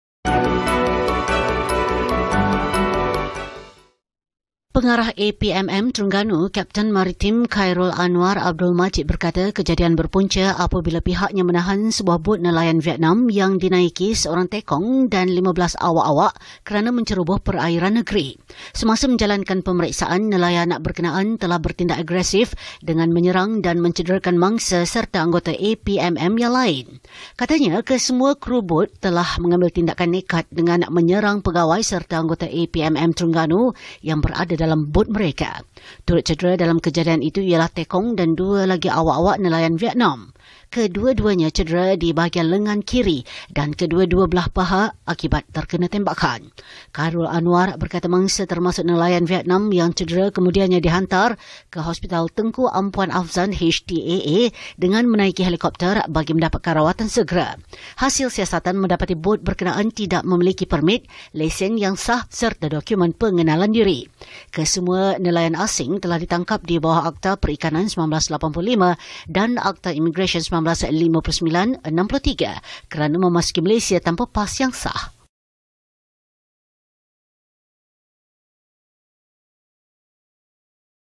Pegawai APMM Terengganu cedera ditetak nelayan Vietnam
BuletinBernama: Seorang pegawai Agensi Penguatkuasaan Maritim Malaysia (APMM) Terengganu cedera parah di kepala selepas ditetak dengan parang oleh nelayan Vietnam.

Ketika kejadian 11.30 pagi tadi, mangsa, Leftenan Muda Maritim Shakir Zufayri Azizul, 27 tahun, sedang menjalankan operasi di perairan bersama 18 lagi pegawai serta anggota APMM di lokasi 148 batu nautika dari Kuala Terengganu.